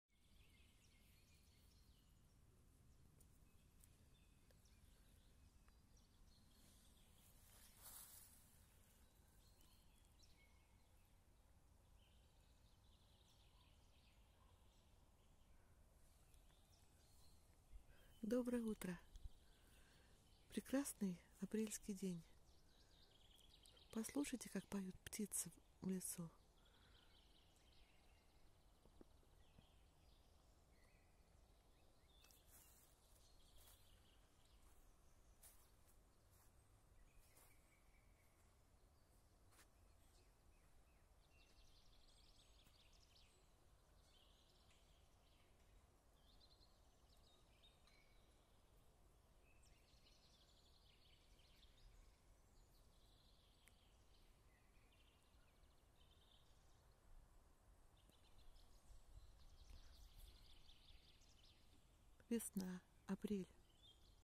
Солнечные лучи в утреннем лесу.
Апрельское утро в  подмосковном лесу. Блестят льдинки после ночных заморозков, встает солнце из-за лесной чащи, поют на все голоса птицы. 2020г.